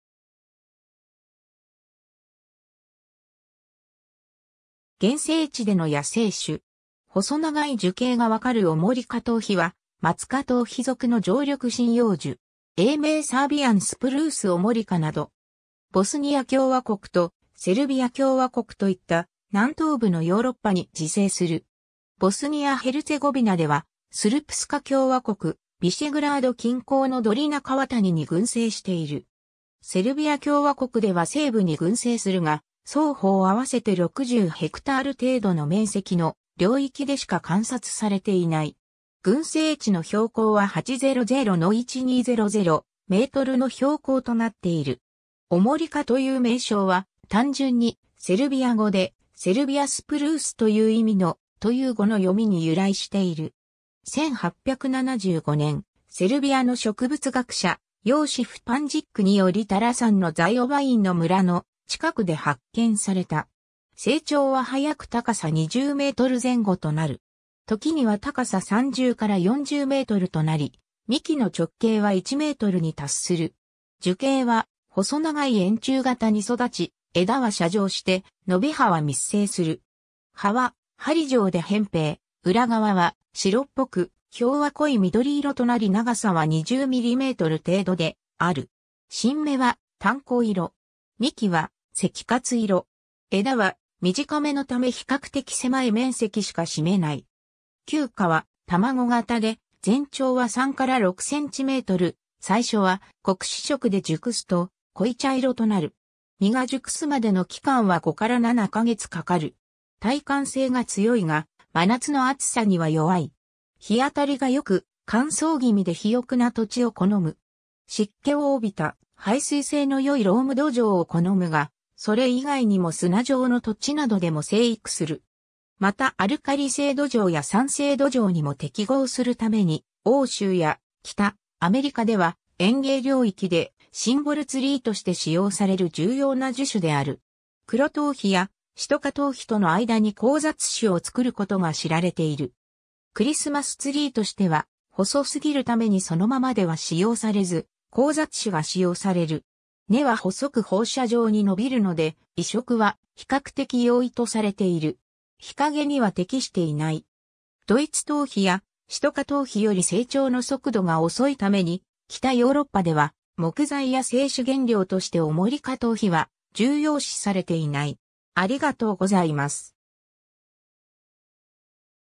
オモリカトウヒ
原生地での野生種。
細長い樹形が判る オモリカトウヒ(学名Picea omorika)は、マツ科トウヒ属の常緑針葉樹。
英名 Serbian spruce; omorikaなど。
 ボスニア共和国とセルビア共和国といった南東部のヨーロッパに自生する。
ボスニア・ヘルツェゴビナでは、スルプスカ共和国ヴィシェグラード近郊のドリナ川谷に群生している。
セルビア共和国では西部に群生するが、双方合わせて60ha程度の面積の領域でしか観察されていない。
群生地の標高は800-1200mの標高となっている。
｢オモリカ｣という名称は、単純にセルビア語で｢セルビアスプルース｣という意味の｢смрча｣という語の読みに由来している。
1875年、セルビアの植物学者Josif Pančič(ヨシフ・パンチッチ)によりタラ山のZaovineの村の近くで発見された。
 成長は早く高さ20m前後となる。
時には高さ30-40mとなり、幹の直径は1mに達する。
樹形は細長い円柱形に育ち、枝は斜上して伸び葉は密生する。
葉は針状で扁平、裏側は白っぽく、表は濃い緑色となり長さは20mm程度である。
新芽は淡黄色。
幹は赤褐色。
枝は短めのため比較的狭い面積しか占めない。
球果は卵型で全長は3-6cm、最初は黒紫色で熟すと濃茶色となる。
実が熟すまでの期間は5-7か月かかる。
耐寒性が強いが、真夏の暑さには弱い。
日当たりが良く、乾燥気味で肥沃な土地を好む。
 湿気を帯びた排水性の良いローム土壌を好むが、それ以外にも砂状の土地などでも生育する。
またアルカリ性土壌や酸性土壌にも適合するために、欧州や北アメリカでは、園芸領域でシンボルツリーとして使用される重要な樹種である。
黒トウヒやシトカトウヒ (w:Picea　sitchensis) との間に交雑種を作ることが知られている。
クリスマスツリーとしては、細すぎるためにそのままでは使用されず、交雑種が使用される。
根は細く放射状に伸びるので、移植は比較的容易とされている。
日陰には適していない。
 ドイツトウヒやシトカトウヒより成長の速度が遅いために、北ヨーロッパでは木材や製紙原料としてオモリカトウヒは重要視されていない。